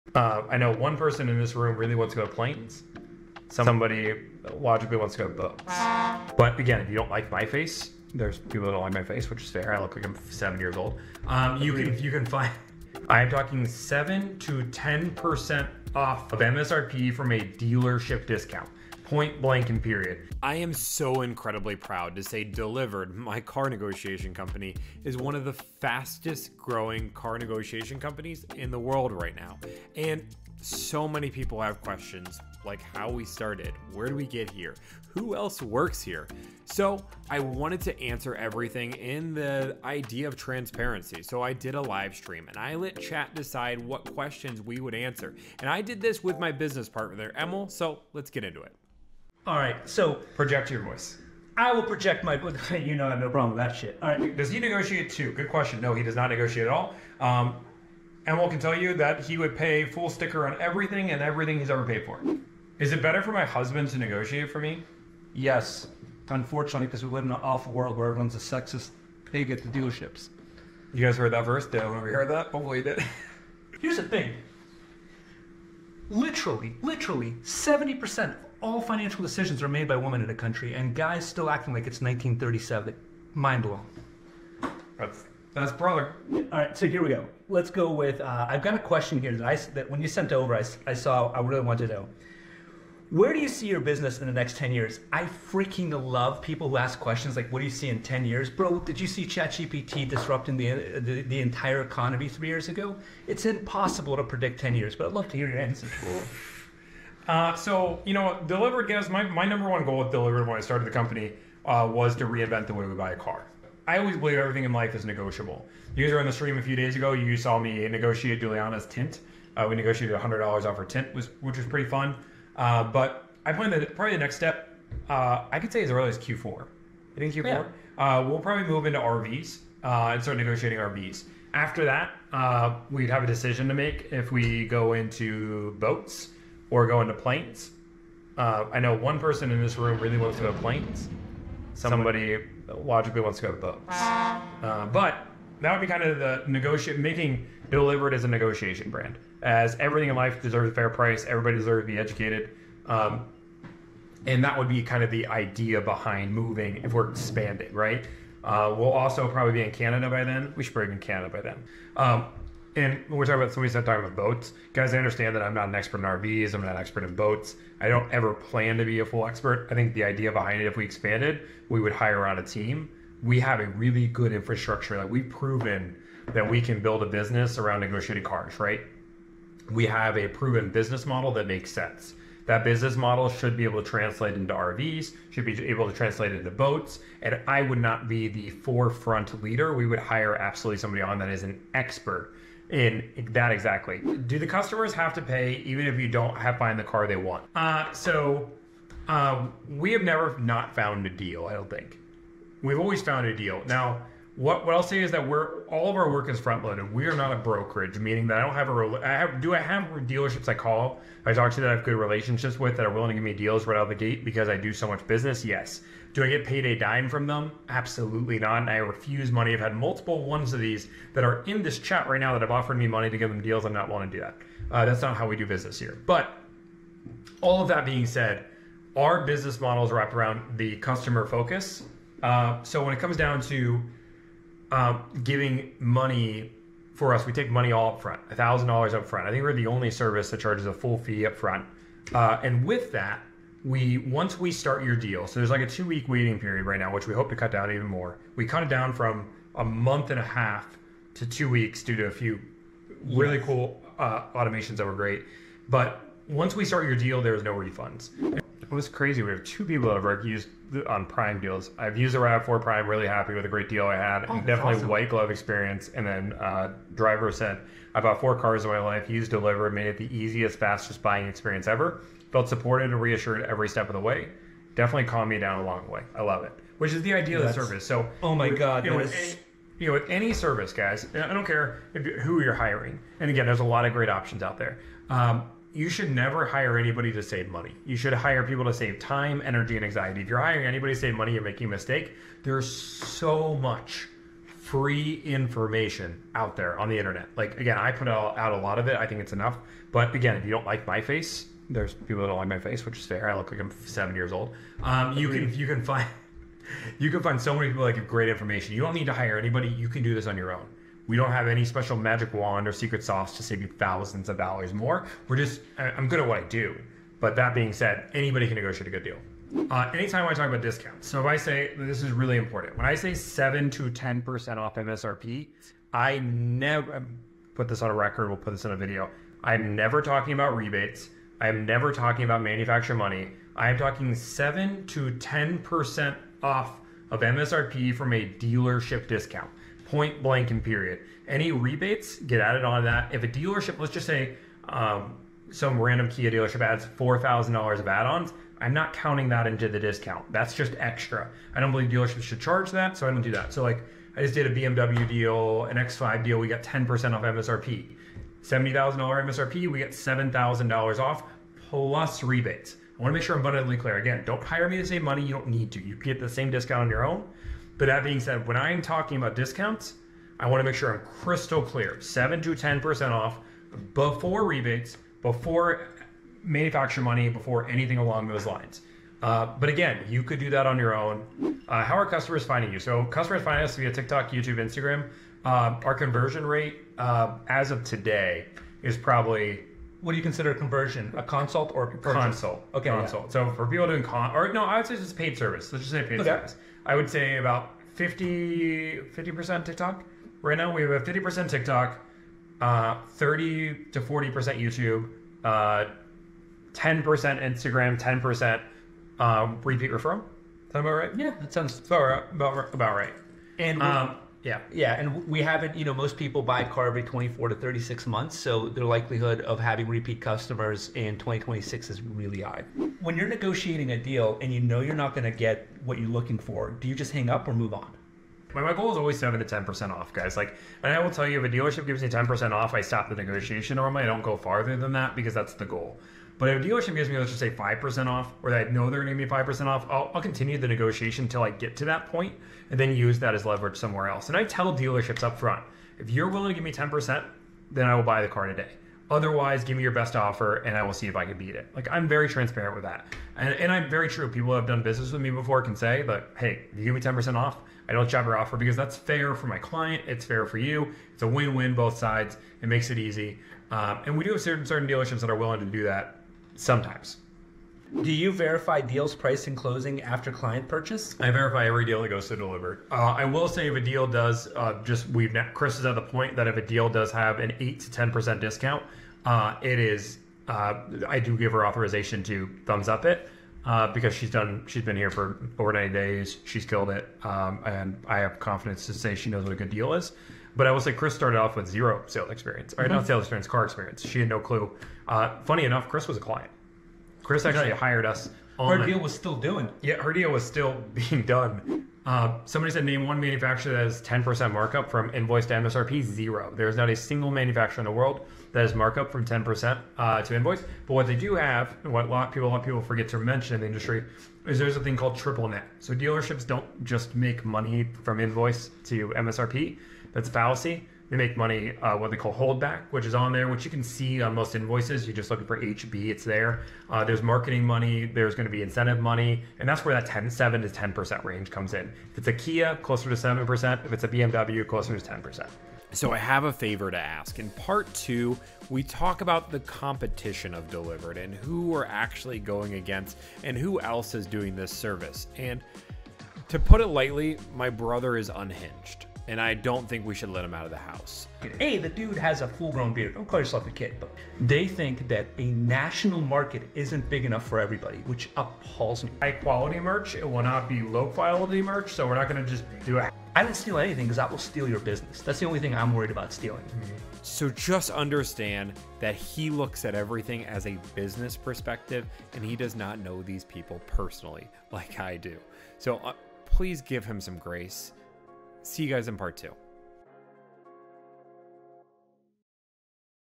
Car Dealerships Freaking Out Over This.......
PART 1 of a 3 part series on a Q/A with the fastest growing car negotiation company in America.  Delivrd.

I negotiate car deals and often do it daily on my live streams. You definitely don't want to miss the action.

📅 Need me to negotiate your next car deal?